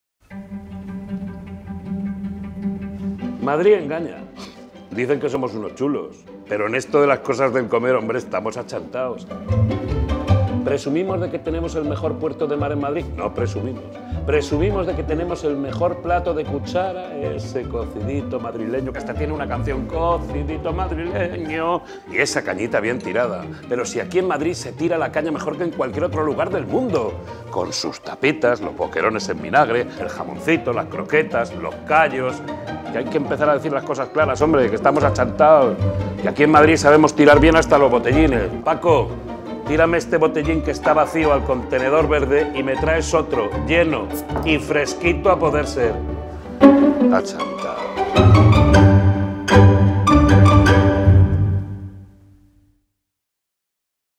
Juan Echanove recicla a la madrileña
Nueva campaña de Ecovidrio con embajadores de excepción